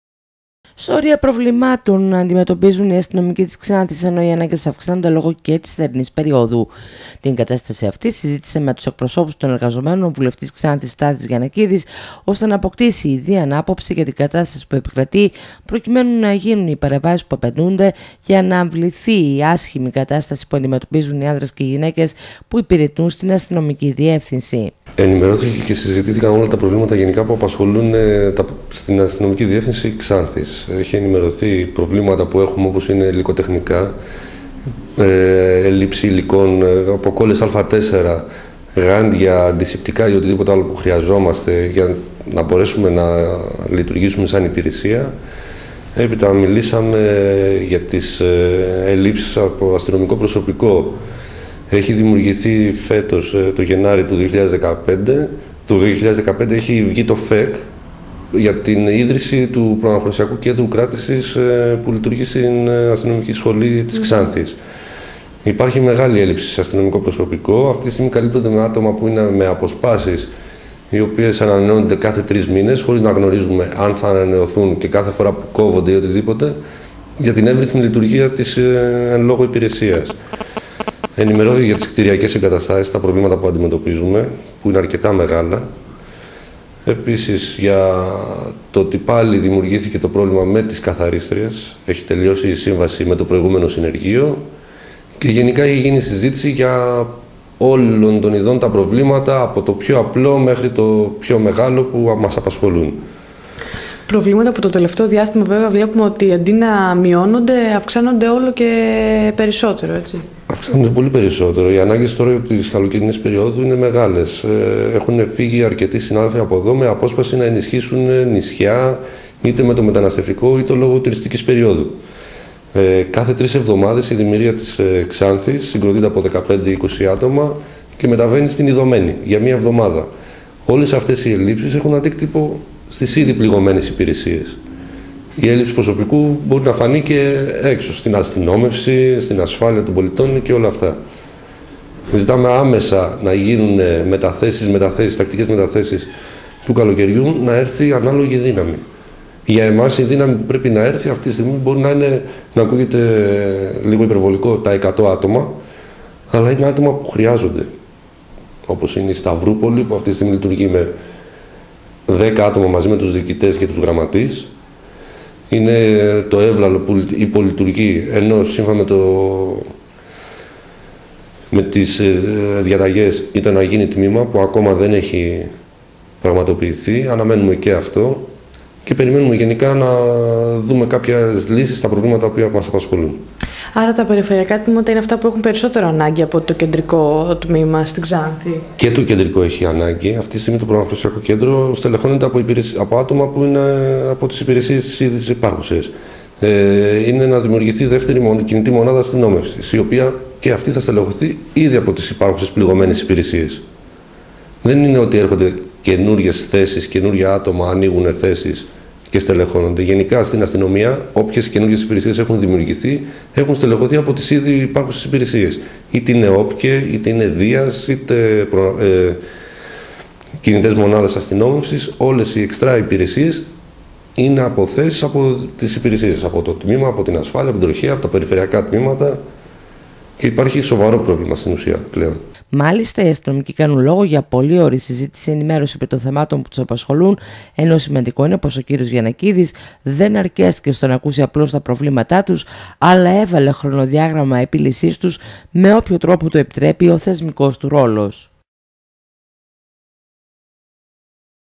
ΠΟΛΥΩΡΗ ΣΥΖΗΤΗΣΗ ΓΙΑΝΝΑΚΙΔΗ ΑΣΤΥΝΟΜΙΚΩΝ ΓΙΑ ΤΑ ΠΡΟΒΛΗΜΑΤΑ ΠΟΥ ΑΝΤΙΜΕΤΩΠΙΖΟΥΝ
ΠΟΛΥΩΡΗ ΣΥΖΗΤΗΣΗ ΓΙΑΝΝΑΚΙΔΗ ΑΣΤΥΝΟΜΙΚΩΝ ΓΙΑ ΤΑ ΠΡΟΒΛΗΜΑΤΑ ΠΟΥ ΑΝΤΙΜΕΤΩΠΙΖΟΥΝ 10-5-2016